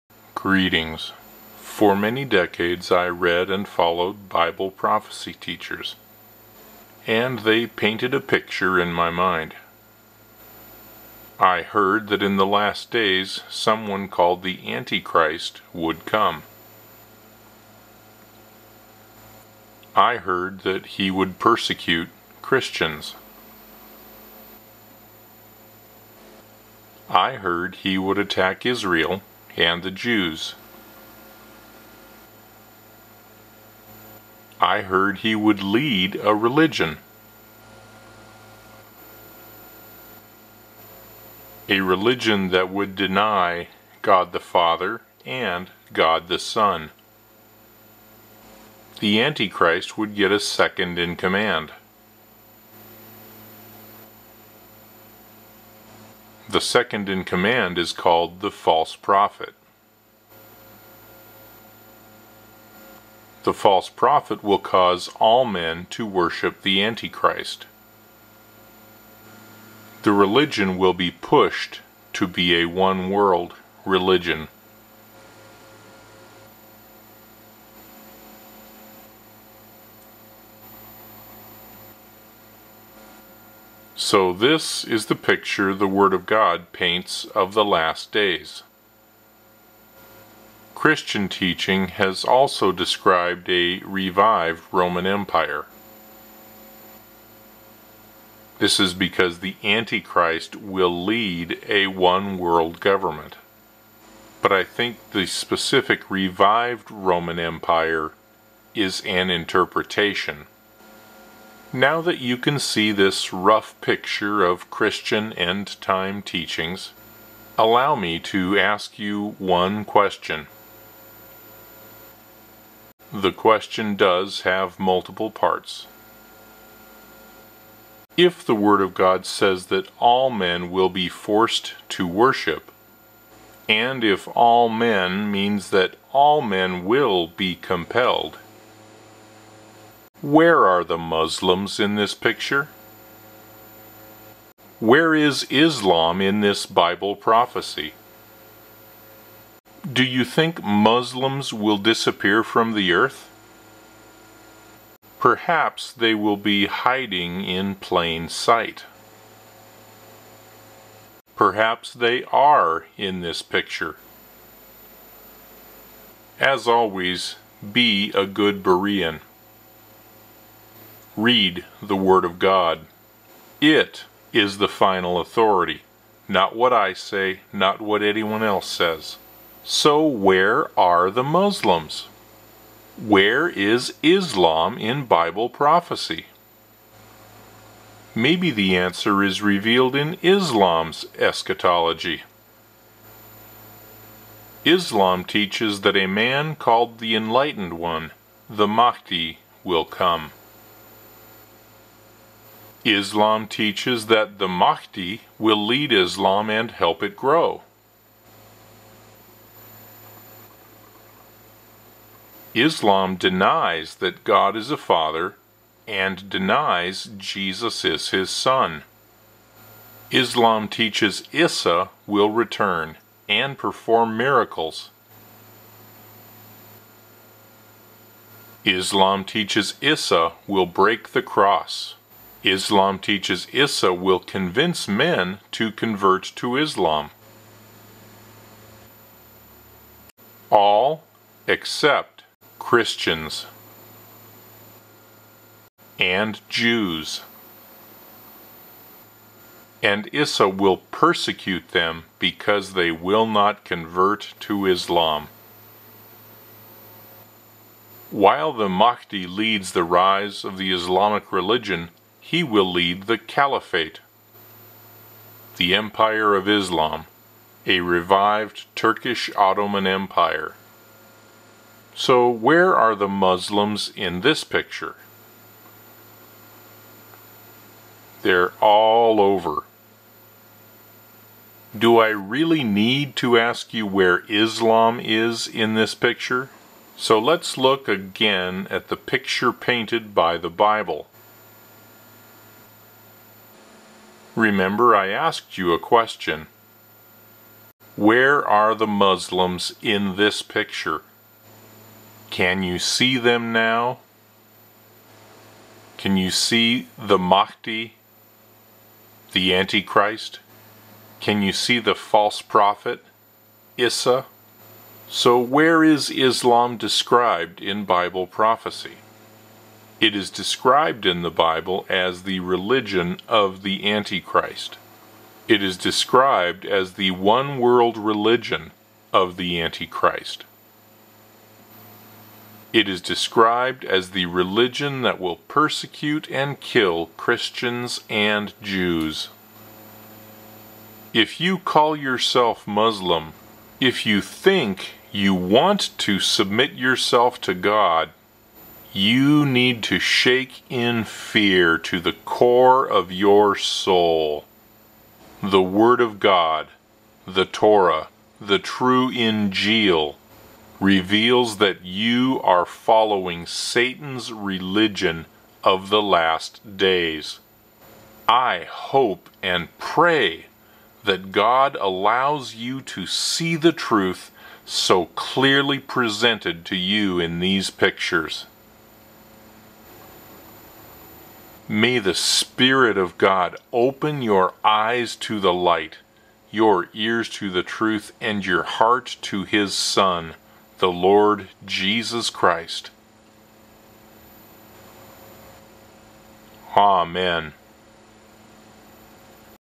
Islam in Bible Prophecy - Where are the Muslims ?
You might be surprised by the similarities exhibited in the pictures of the Christian End Time Prophecy and the Islamic Eschatology.

I made this video because there was a question that always came to mind, the more and more I though about ISLAM in Bible Prophecy.